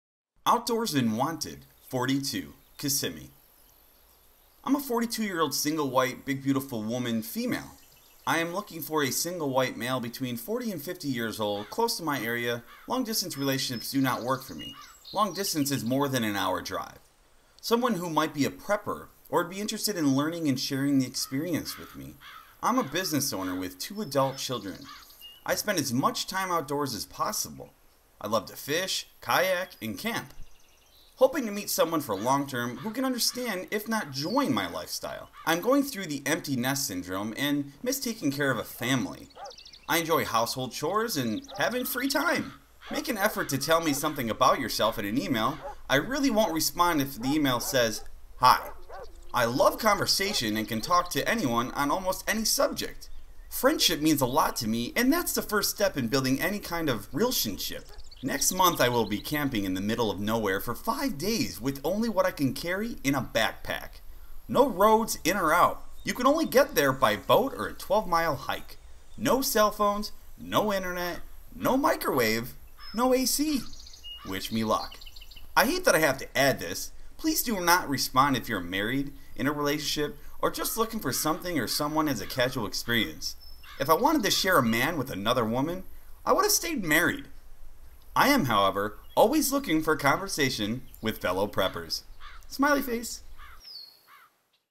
Outdoorsman Wanted - 42 (Craigslist Personals)
This is a REAL Craigslist post I found in the Personals section.

I am a 42 year old single white bbw female. I am looking for a SWM between 40-50 years old, close to my area, long distance relationships do not work for me. Long distance is more than an hour drive. Someone who might be a prepper or would be interested in learning and sharing the experience with me. I am a business owner with two adult children. I spend as much time outdoors as possible. I love to fish, kayak, and camp. Hoping to meet someone for long term who can understand if not join my lifestyle. I am going through the empty nest syndrome and miss taking care of a family. I enjoy household chores and having free time. Make an effort to tell me something about yourself in an email, I really wont respond to an email that says "hi". I love conversation and can talk to anyone on almost any subject. Friendship means a lot to me and thats the first step in building any kind of realtionship.

Next month I will be camping in the middle of no where for five days with only what i can carry in a backpack. No roads in or out, you can only get there by boat or 12 mile hike. No cell phones, no interenet, no microwave, no a/c. Wish me luck.

FINE PRINT
I hate that I have to add this. Please do not respond if your are married, in a relationship or just looking for something or someone as a casual experience.If i wanted to share a man with another woman I would have stayed married. I am however always looking for conversation with fellow preppers:)